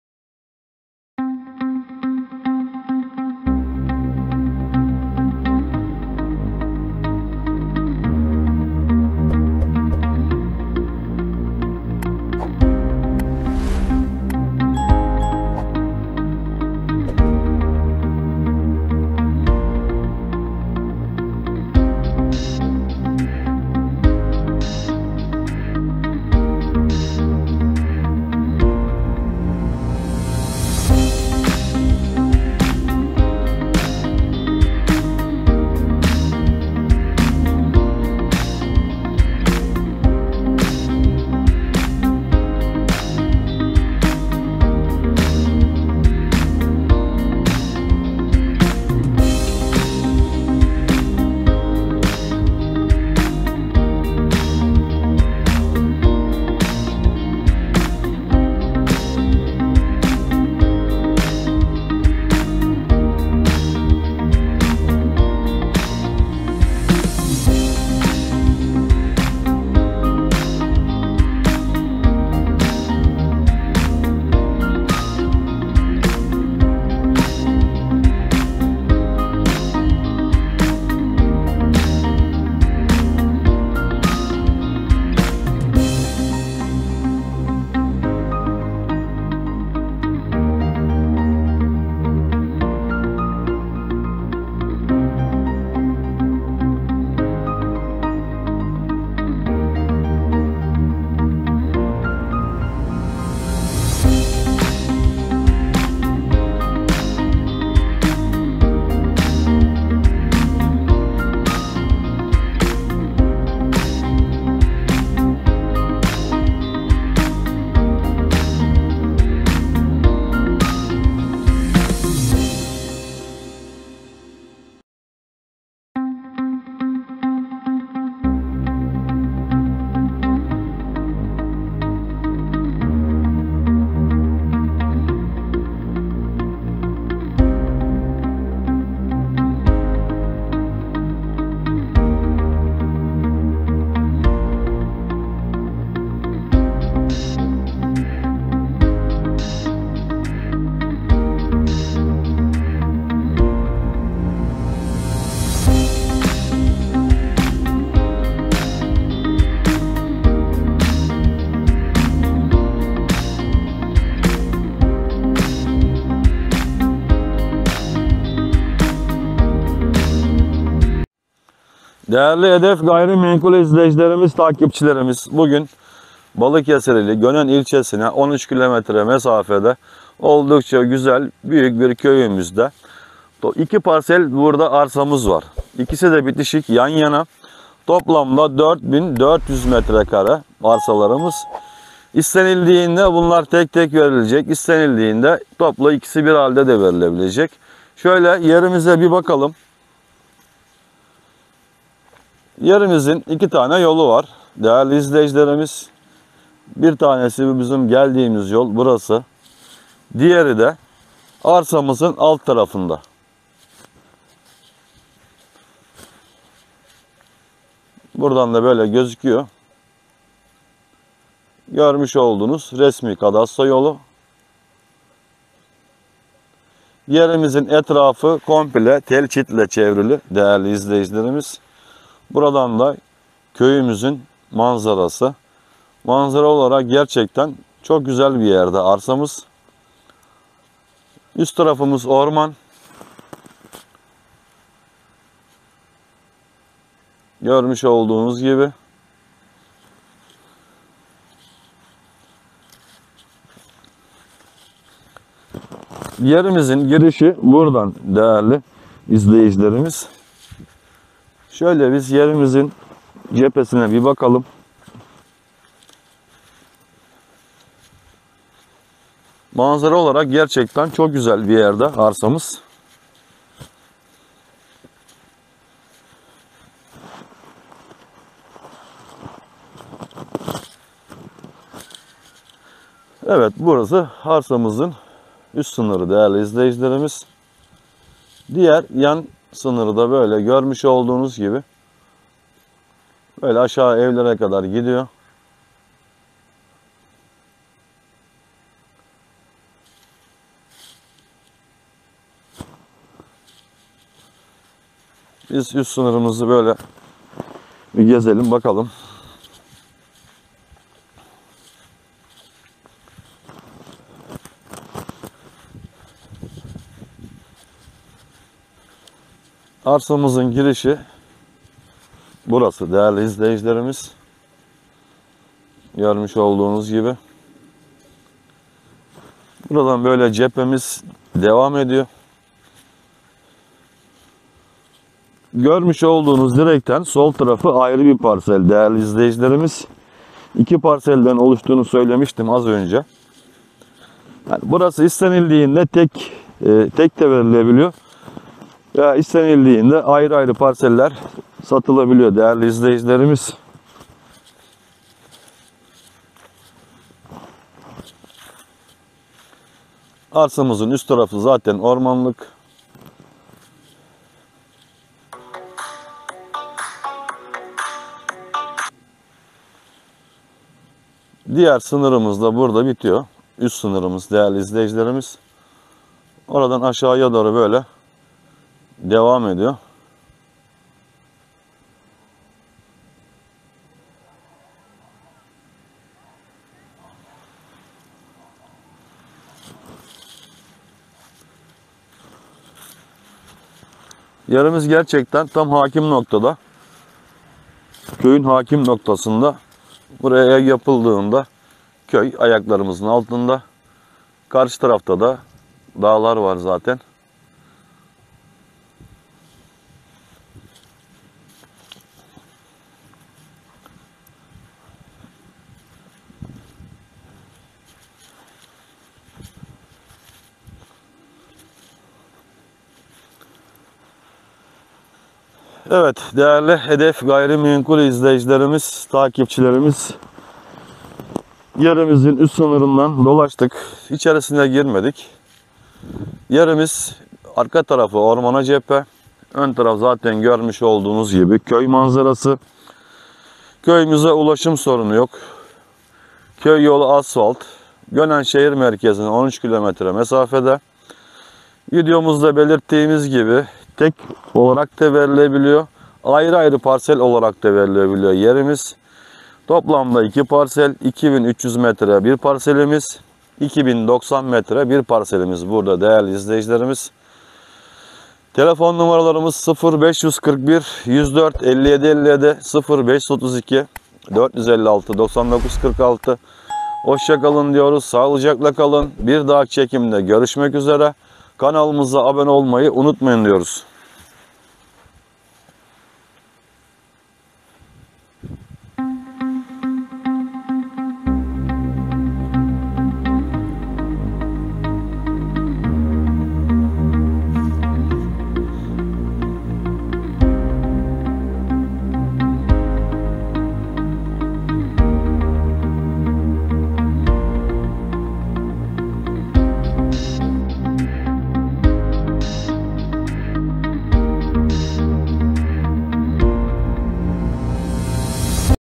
179 GÖNEN'DE DOĞA İLE İÇ İÇE 2.300 M2.+2.100 M2. ARSA/İSTENİRSE TEK TEK SATILABİLİR
FİYAT:2,300 M2. OLAN ARSA 1,050,000 TL.
           2,100M2. OLAN ARSA 960,000 TL
GÖNEN'DE DOĞA İLE İÇ İÇE 2.300 M2.+2.100 M2. ARSA/İSTENİRSE TEK TEK  SATILABİLİR

MUSTAFA ARSLAN - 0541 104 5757
KAHRAMAN TRABZON - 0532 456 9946